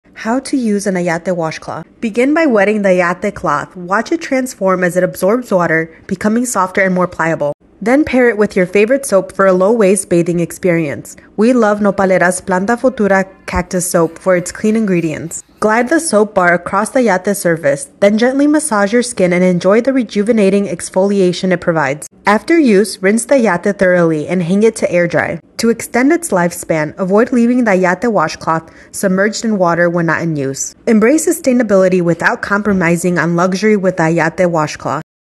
Everything to Know about Ayate Washcloths - What it is, how to use it and how to care for it.
Ayate is a natural fiber that comes from the maguey plant – the plant from which tequila and mezcal are produced.

Ayates have been used for centuries in Mexico. The exact origin date is not well documented, but they have a long history deeply rooted in indigenous communities.

Traditionally, ayate cloths were used by indigenous communities for various purposes, including personal hygiene, exfoliation, and household cleaning.

The ayate crafting tradition is intricate, labor intensive and has been passed down over generations, especially in areas like Central Mexico where agave plants grow natively.

Despite its rich heritage, ayate production has dwindled over time. They've been replaced by the convenience of plastic, to the detriment of the planeta.

Thankfully, there are still a handful of families in Central Mexico that continue the ayate crafting tradition.

We've partnered with a cooperative that makes ayates the traditional way, hand-spun using sustainably-sourced ixtle (agave) fibers. We partner with them using fair trade principles to bring you these ancestral bathing accessories.

Ayate washcloths feel coarse when dry, but they soften for the perfect amount of exfoliation once they're wet.

They are very resistant to mold, which makes them a perfect, better alternative to plastic loofahs and washcloths.

Plus, they're fully biodegradable and compostable.

We've made it so easy for you to ditch all of your plastic exfoliating accessories, while honoring our ancestors' tradition of ayate washcloths. Watch this video to learn how to use one. This is your sign to finally ditch your plastic loofah!